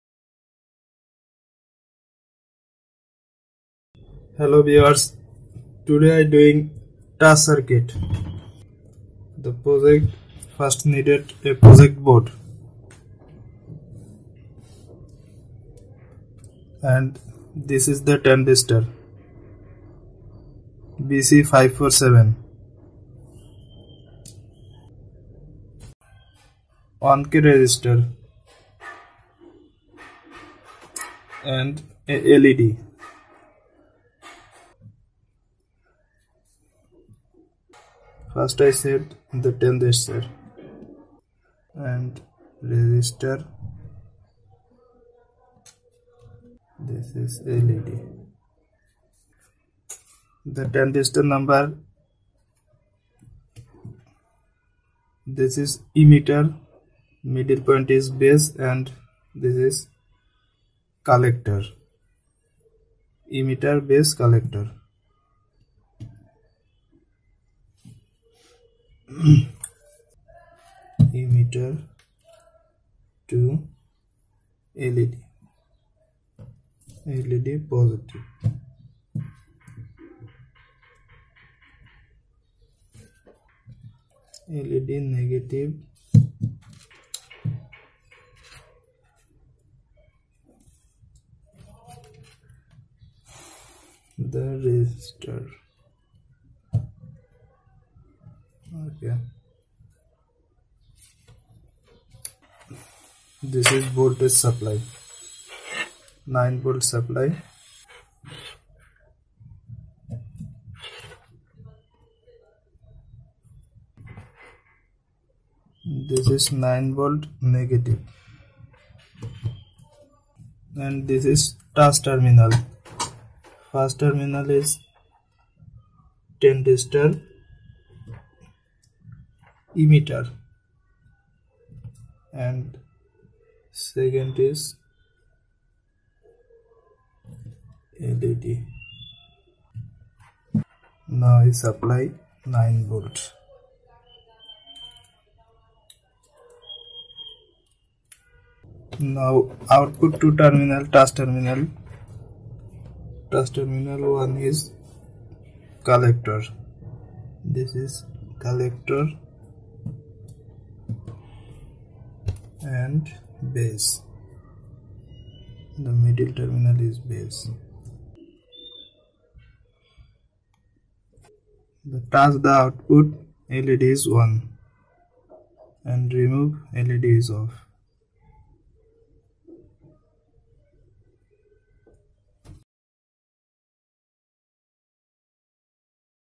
BC547 Transistor ON Touch Switch Circuit Earthbondhon
Components Required:
BC547 (or any other NPN transistors)
Resistors: 1KOhm
LED
Breadboard Connectors
Breadboard
9V Power Supply